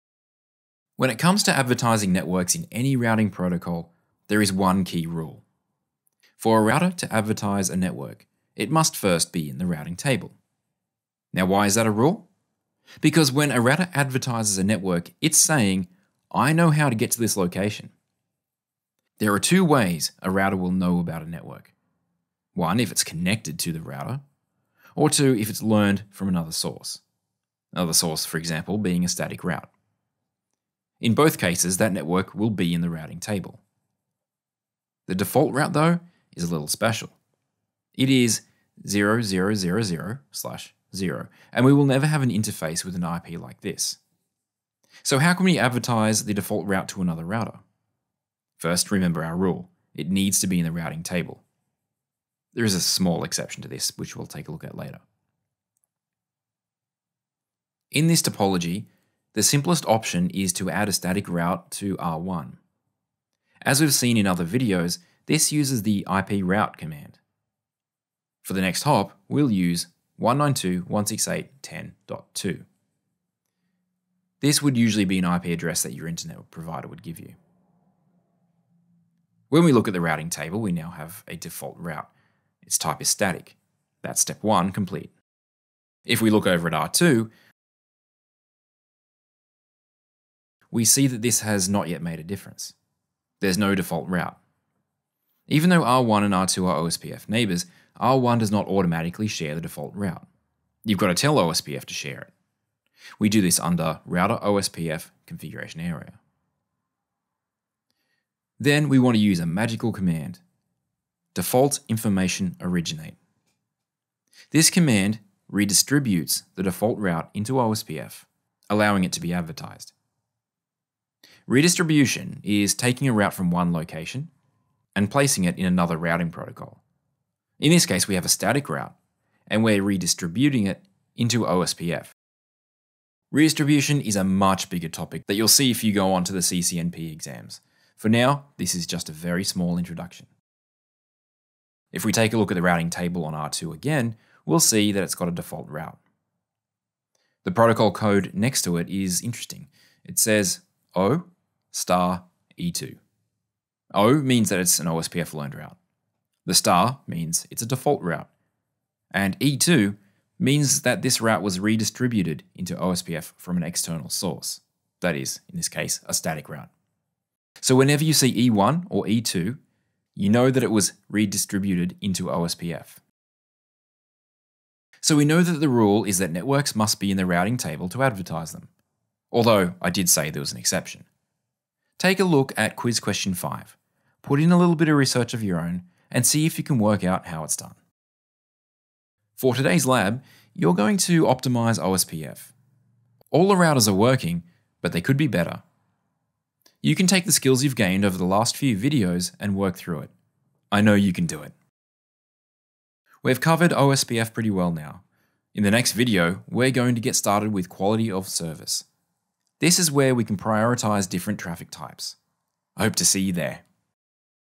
OSPF Default Route | Cisco Configuration
How can we advertise the default route to another router using OSPF?
Learn the answer to this question and more about OSPF (Open Shortest Path First) in this video.